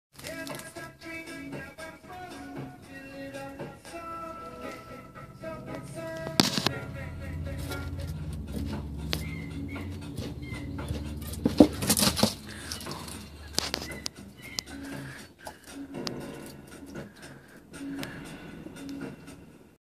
Jumping for horn wroms
Ari super excited to eat a horn worm. She warms up her legs and takes a big jump. I have never see her do this before, I honestly though she was just going to reach her tongue out.